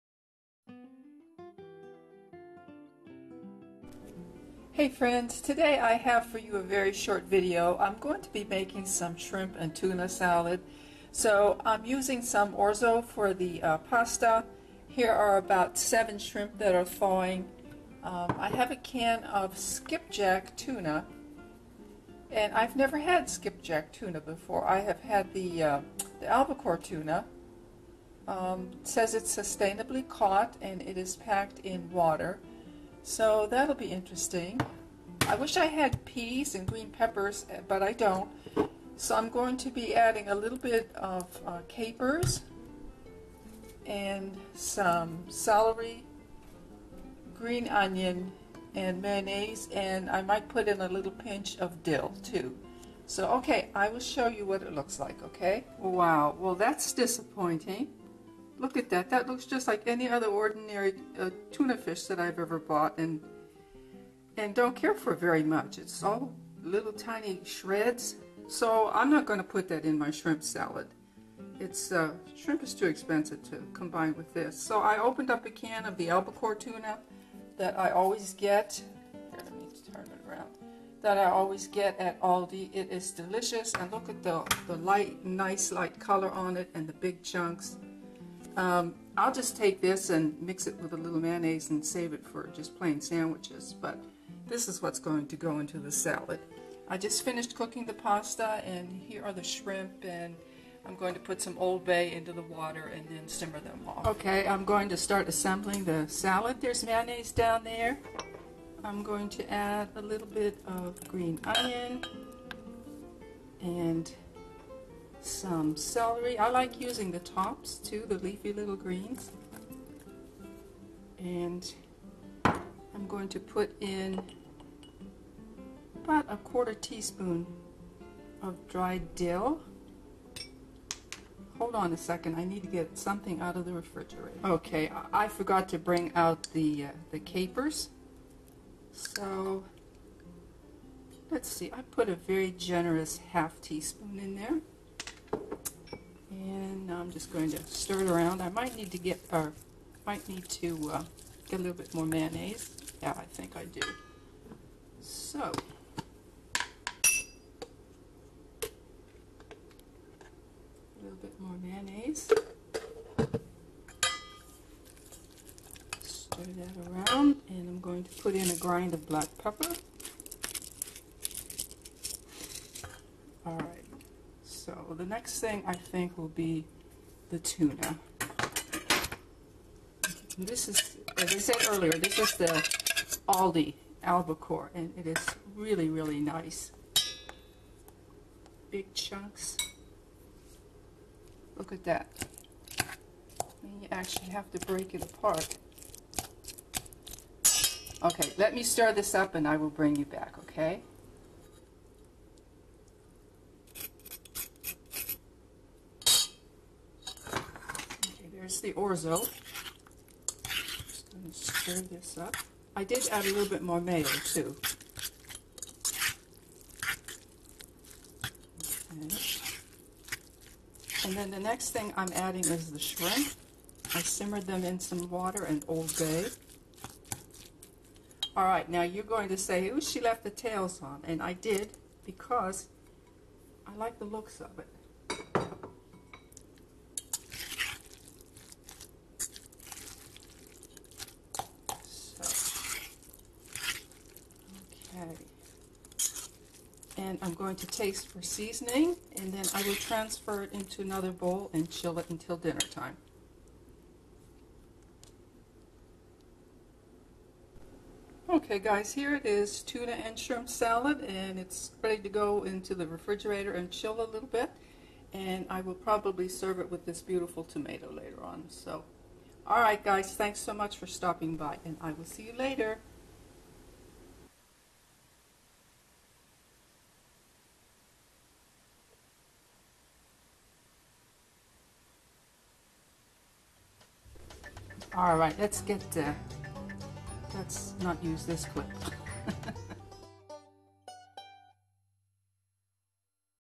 ~ cooking for one: tuna salad with shrimp ~
I love shrimp almost any old way, including this refreshing and easy salad with tuna and orzo. What are some of your favorite summer salads?